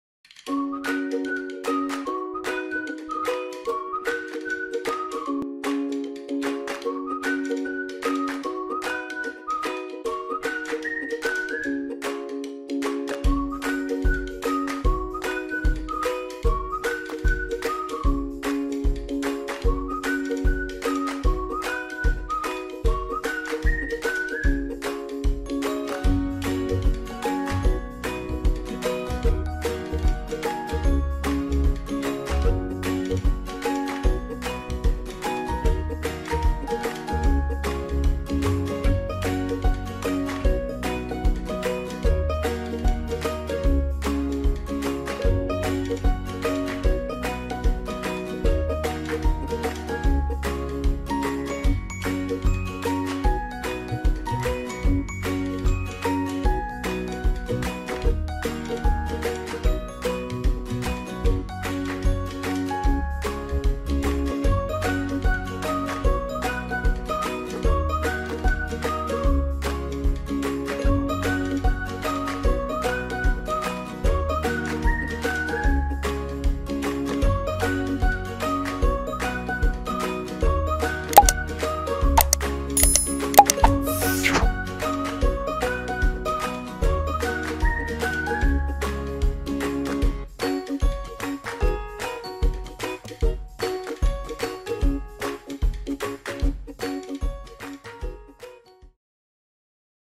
Quick Shahi Kulfa Recipe/How to make Shahi Kulfa at home recipe/Easy Pakistani Shahi Kulfa Recipe
This is a quick and easy recipe to make Shahi Kulfa at home. The ingredients of this Shahi Kulfa are easily available.